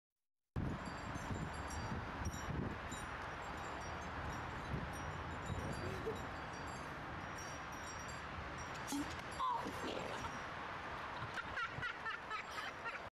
Danny gets shot with a Nerf Gun!
So, we were over at the neighbor kid's house, and one of the kids there had this giant nerf gun, and like, the nerf bullets' diameter was close to that of a baseball; they were about a foot long. [Well. I guess as you can see in the video]

Neighbor-kid insisted we shoot him with it, and record it. So we did. xD